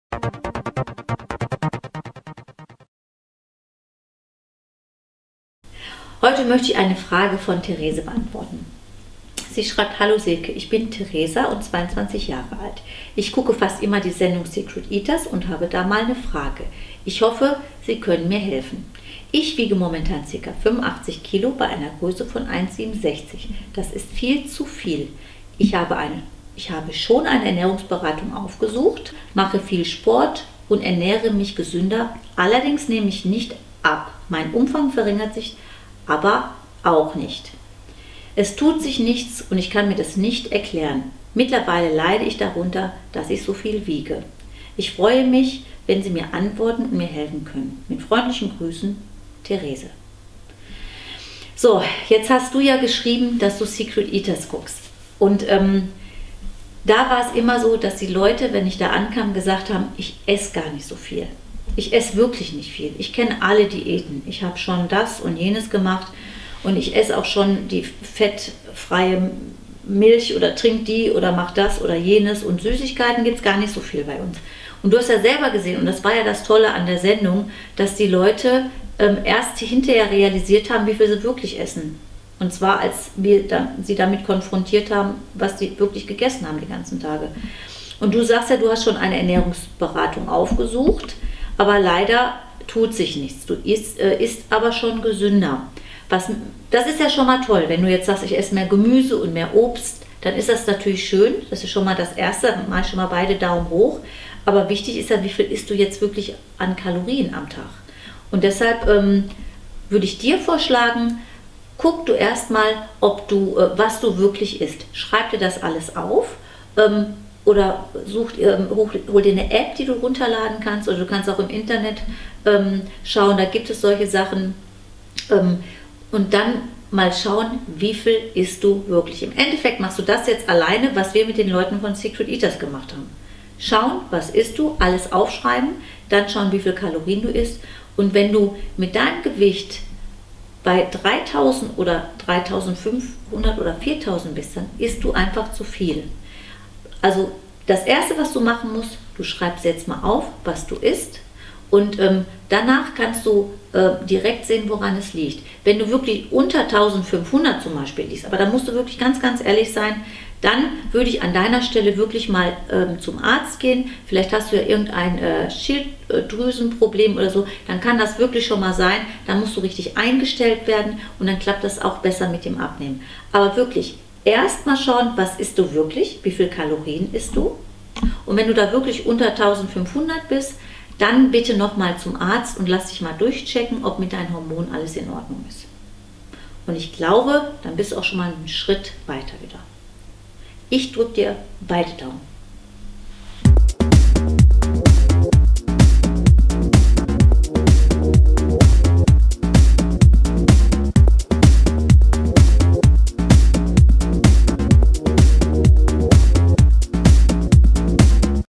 Frage von Theresa: Nach Ernährungsumstellung weniger Umfang, aber gleiches Gewicht
Was macht Theresa falsch und wo ist der Hacken? Silke klärt auf.

App für das Handy und Tablet "das 10 Minuten Workout mit Silke Kayadelen:

Studio in Velbert
Silke Kayadelen
Fit und Gesund
Friedrichstraße 294
42551 Velbert